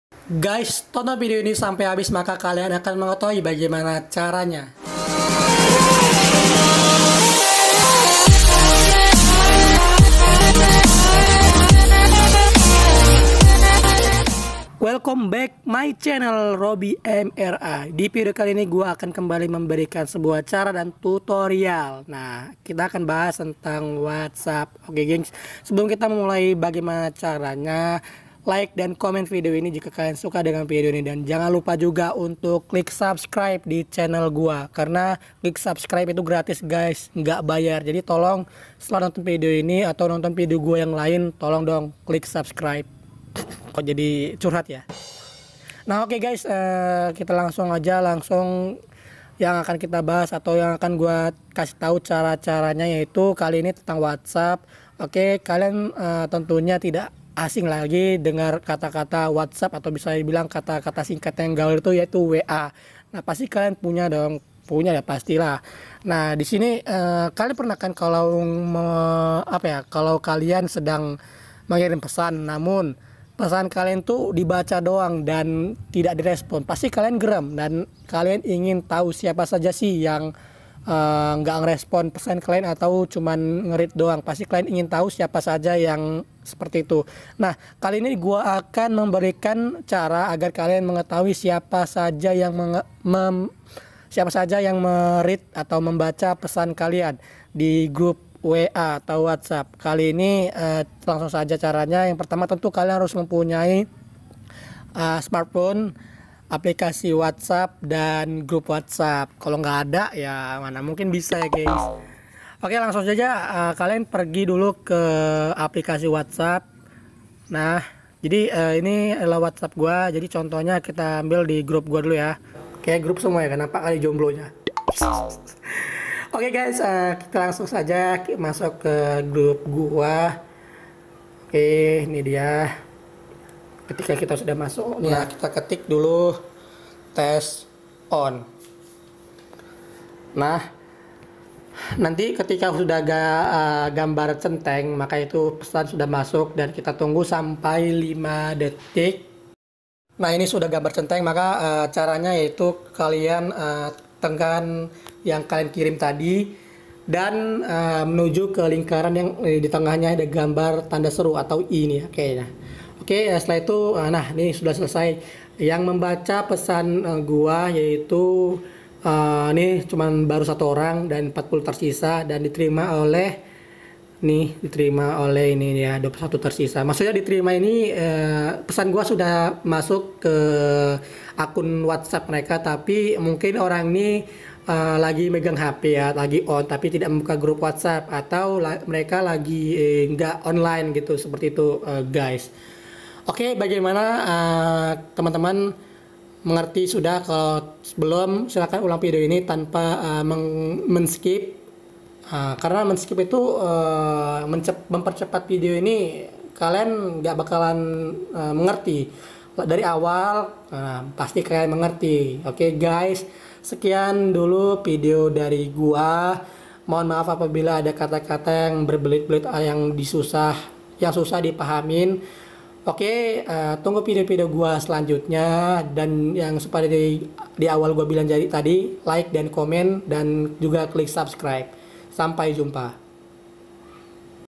Cara melihat siapa saja orang yang membaca pesan kita di group Whatsapp
Intinya kalian semua pernah dong ketika mengirim pesan di Group Whatsapp namun Pesan kalian itu cuma di Baca doang sama teman-teman kalian tanpa di respons sama sekali, dan seketika kalian geram dan ingim mengetahui siapa sih orang ya cuman membaca tanoa merespon nya, nah maka dari itu ini dia solusi dari permasalahan kalian itu tadi gaes, Tonton video ini sampai selesai  oke..